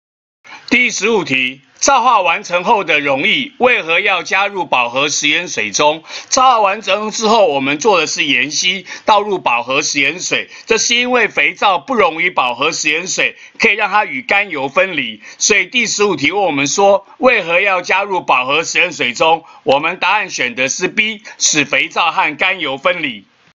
105學年下8年級理化三段第15題
14～17題題組。
問皂化後，為何加入飽和食鹽水中？